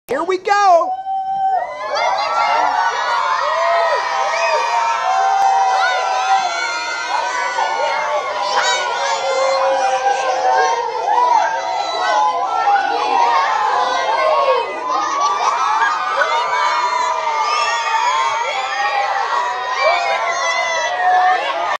WBTV's Eric Thomas teaches kids to stay safe
MONROE - WBTV's Chief Meteorlogist Eric Thomas visited nearly 100 second graders at Wesley Chapel Elementary School during the last week in May.
They had a great time and Eric even taught them how to stay safe in the summer time.
Eric likes to teach kids about thunderstorm and lightning